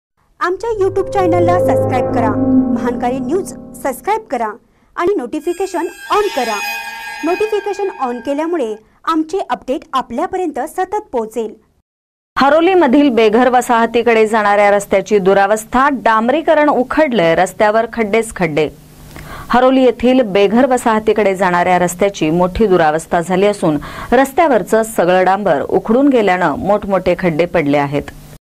हरोली मधील बेघर वसाहतीकडे जाणा-या रस्त्याची दुरावस्था
हरोली मधील बेघर वसाहतीकडे जाणा-या रस्त्याची दुरावस्था : डांबरीकरण उखडलंय, रस्त्यावर खड्डेच खड्डे